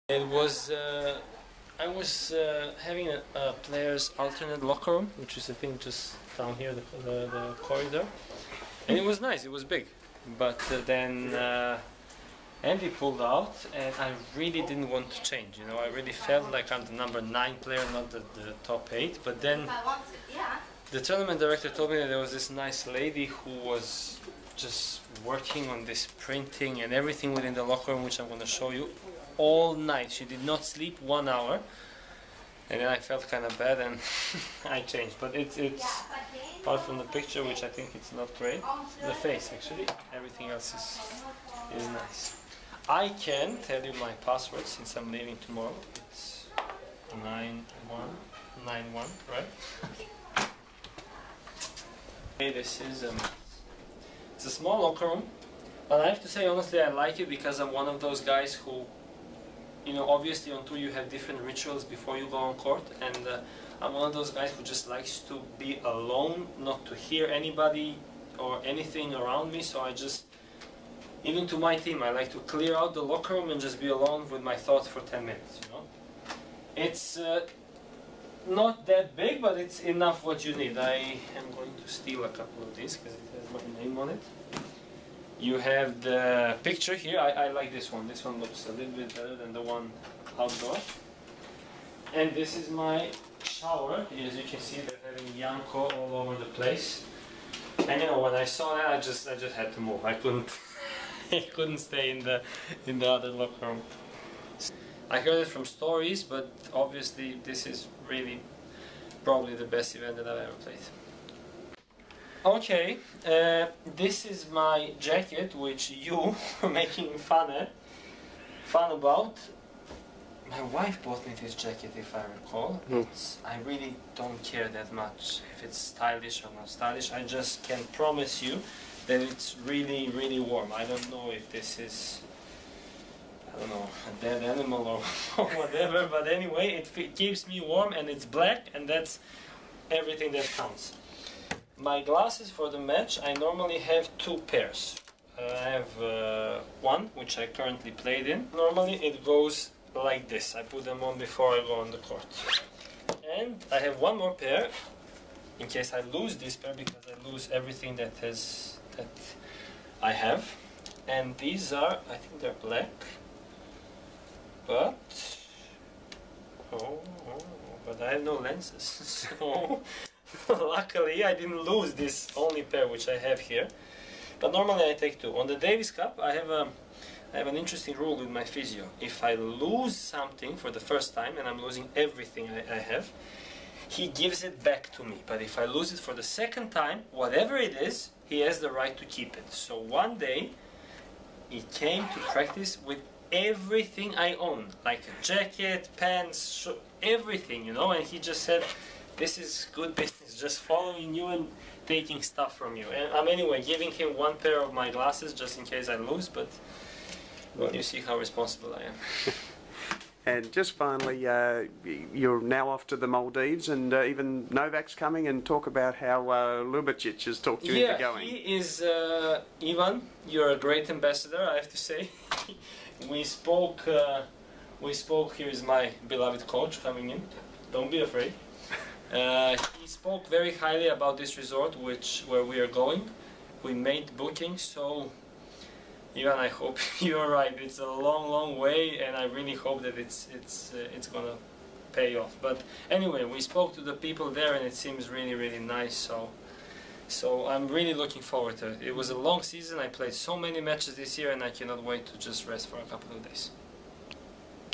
Tipsarevic Takes You Inside His O2 Locker Room
Janko Tipsarevic takes you inside his locker at The O2 and reveals a special rule he has about losing personal belongings.